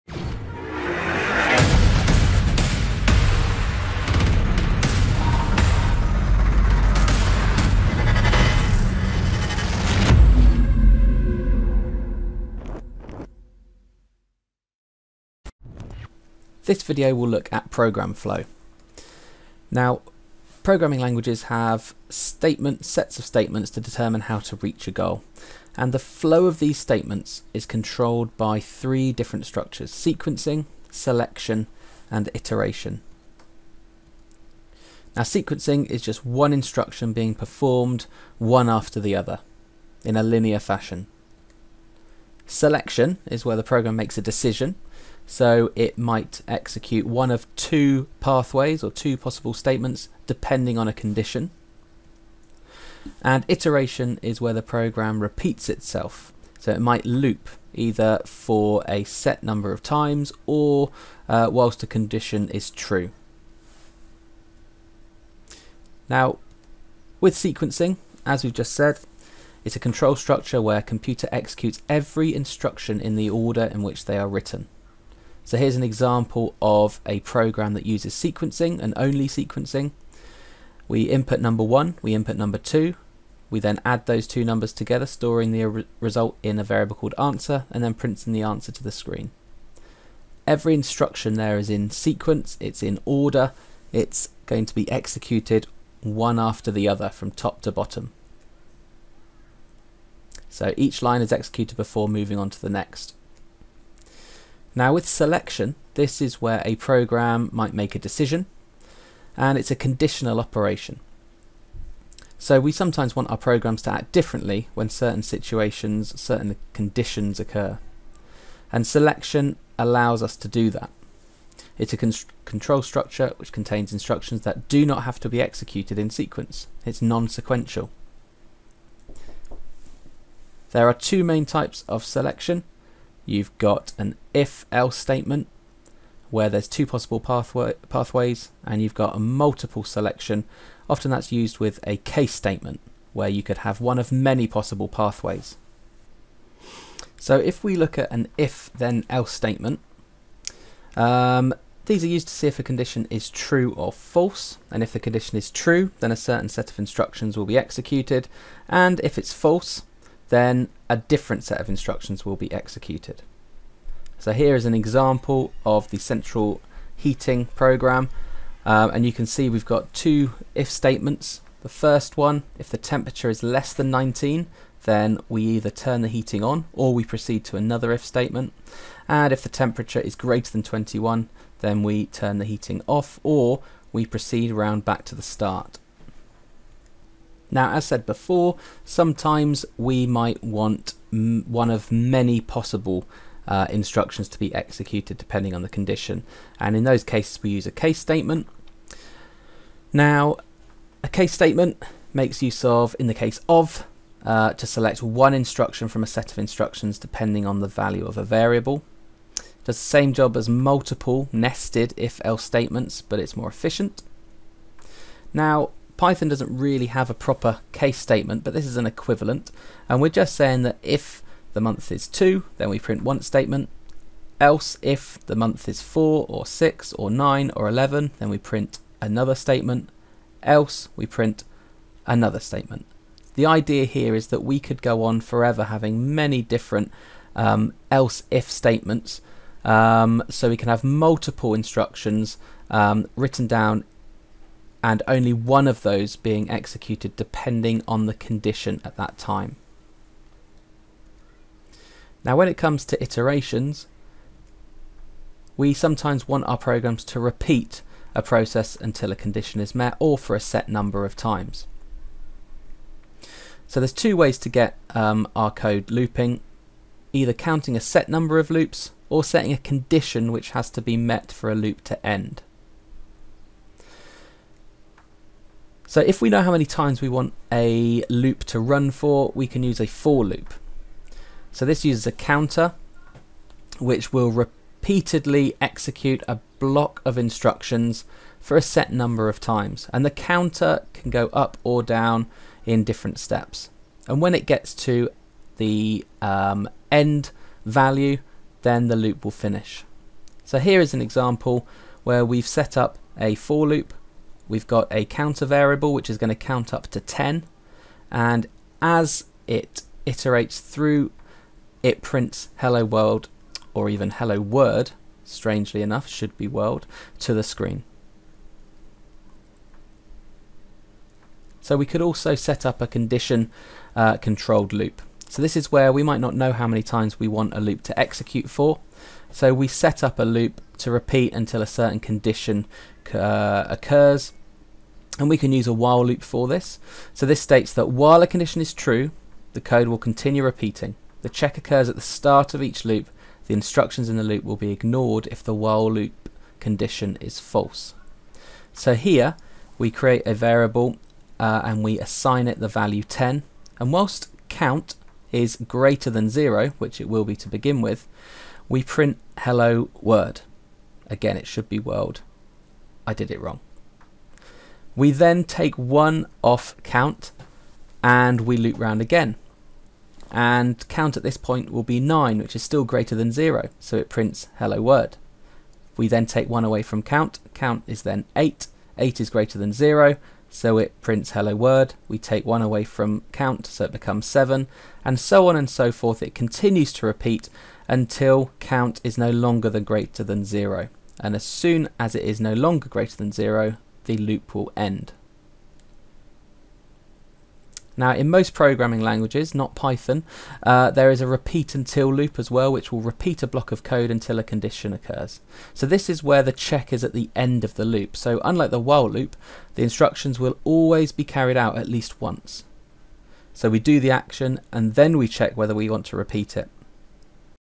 08 Program Flow - GCSE Computer Science
This clip looks at the three main constructs in procedural programming languages (‘Sequencing’, ‘Selection’ and ‘Iteration’) and explains the need and purpose of each, in program code.